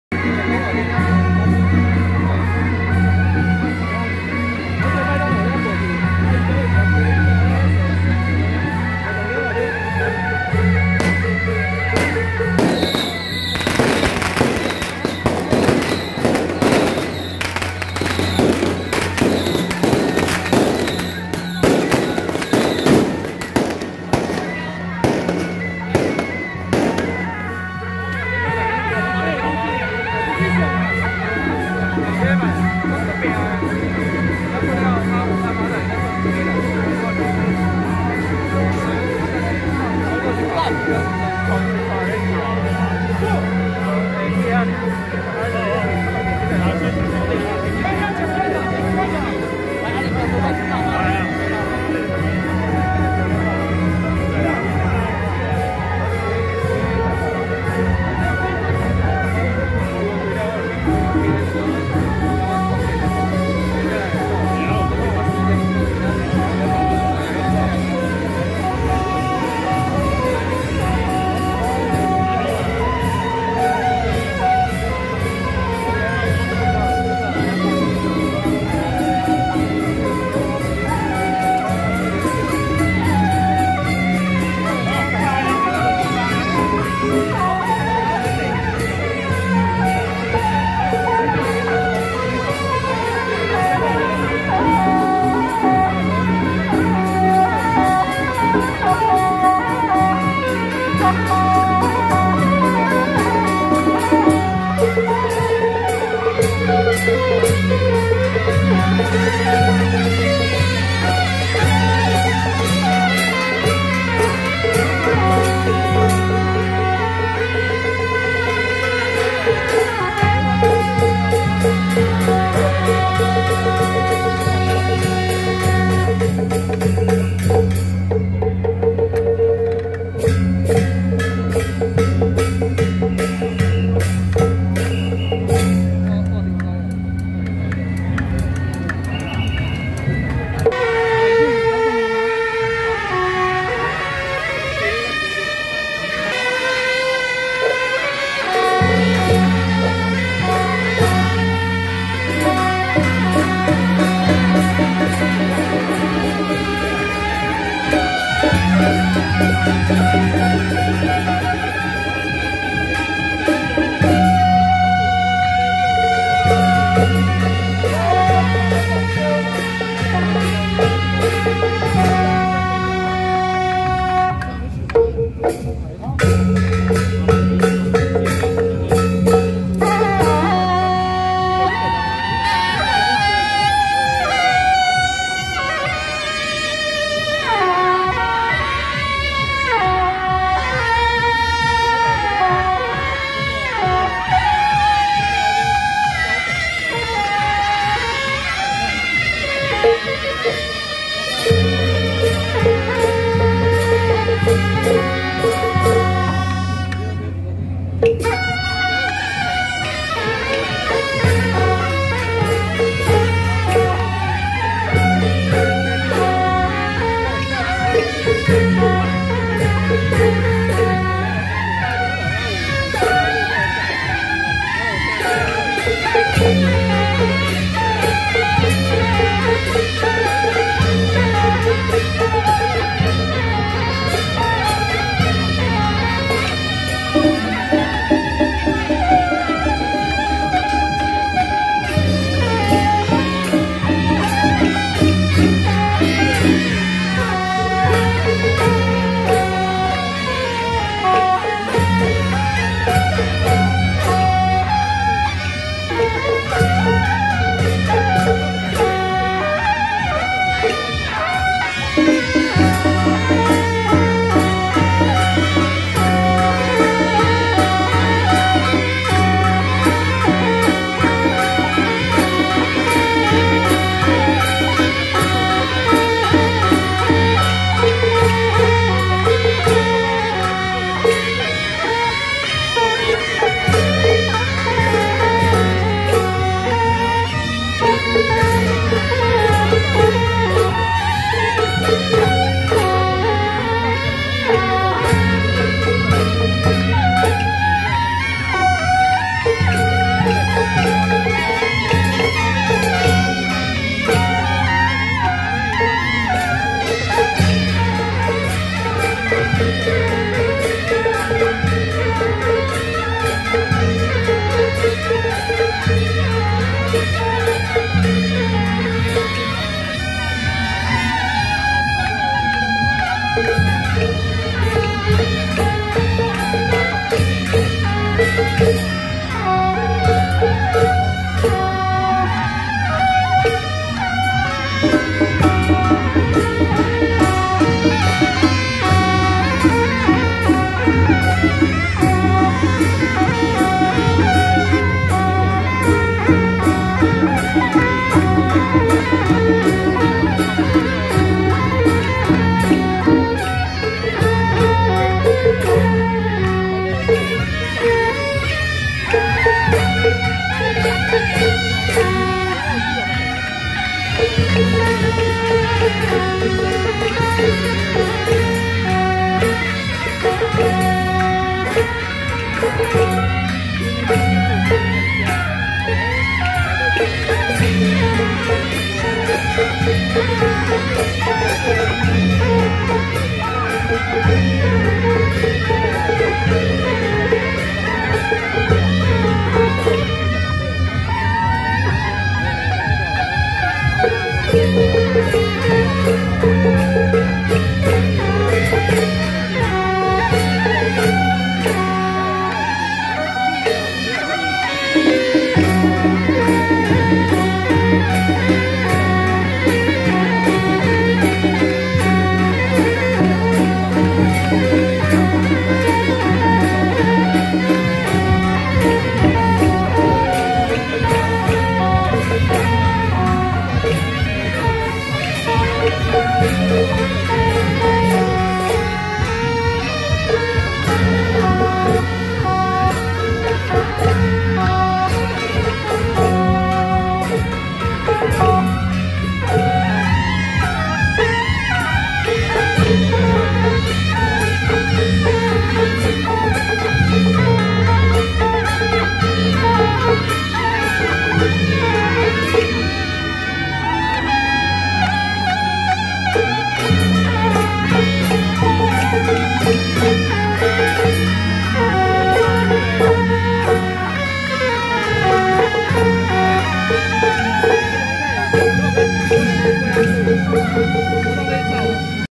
【基隆白米甕帝君會-神將團】~111瑞芳得意堂恭祝田都元帥聖誕暨建堂100週年慶遶境
羅東嘉興團-北管＆基隆白米甕帝君會-神將團